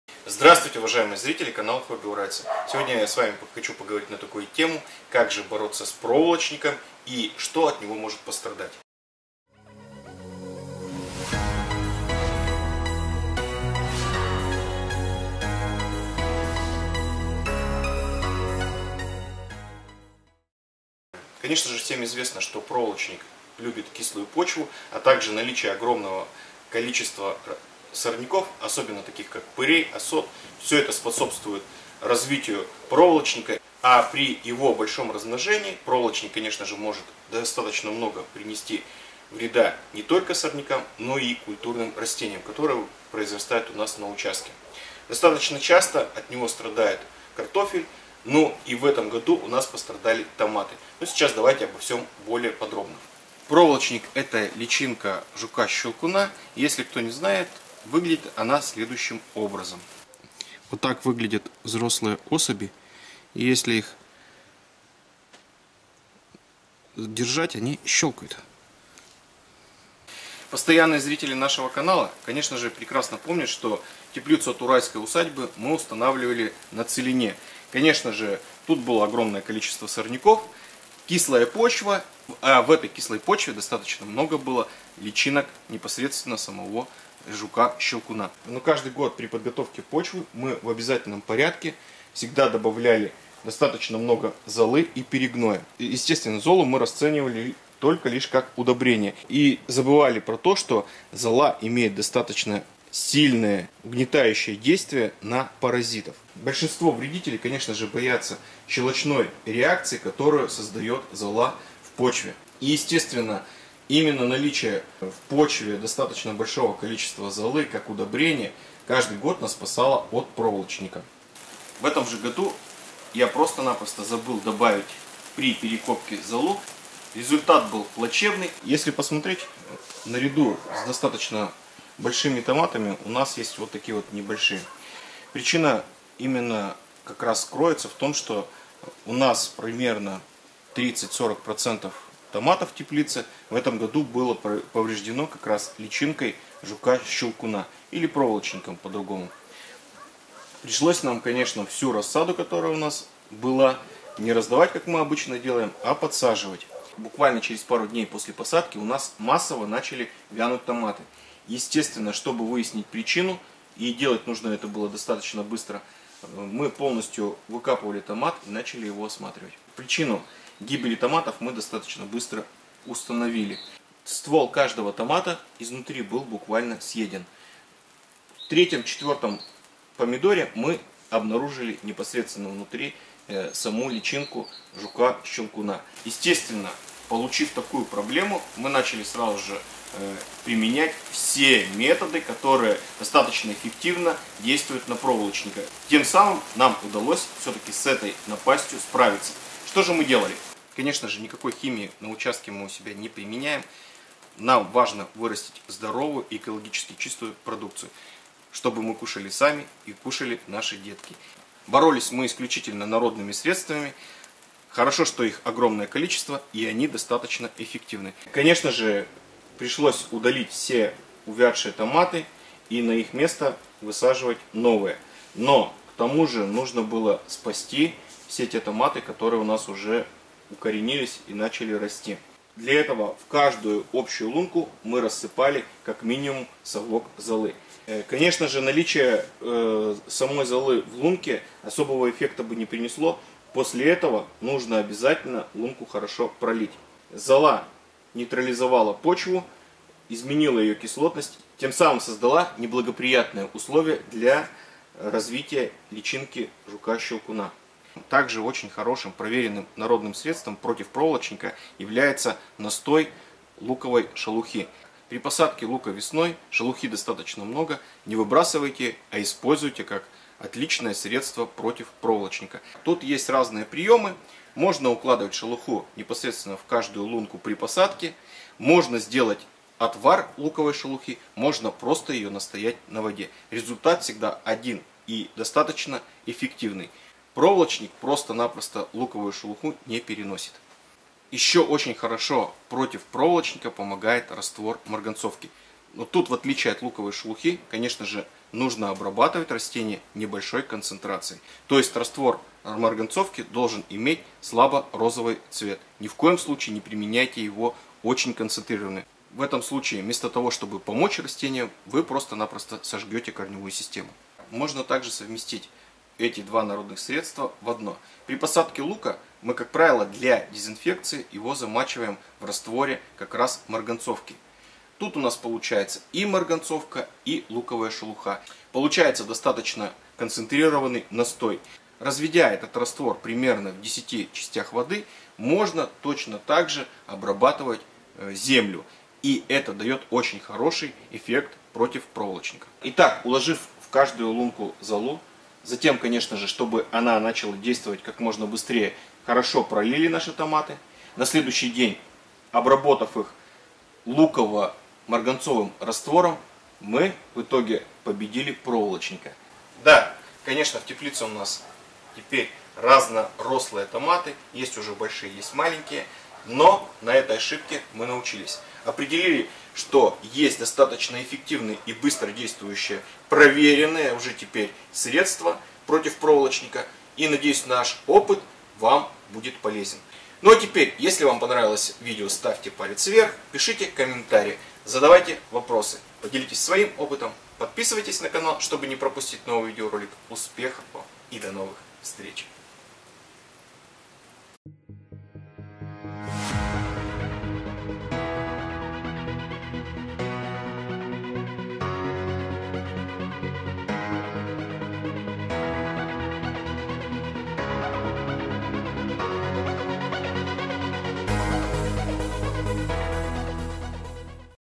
КАК БОРОТЬСЯ С ЖУКОМ-ЩЕЛКУНОМ (ПРОВОЛОЧНИК)? САД (ОГОРОД) И ТЕПЛИЦА
Проволочник или жук-щелкун поражает корнеплоды многих овощей, но может так же буквально выгрызть ствол у томатов изнутри. Как же простыми народными методами избавится от такого вредителя, как проволочник (личинка жука-щелкуна)? проволочник жук-щелкун
НОВЫЕ ВИДЕО КАЖДОЕ ВОСКРЕСЕНЬЕ
Для поддержки канала карта СБЕРБАНКА: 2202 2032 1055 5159
Абсолютно любая сумма окажет поддержку нашему каналу. Заранее, огромное спасибо!!!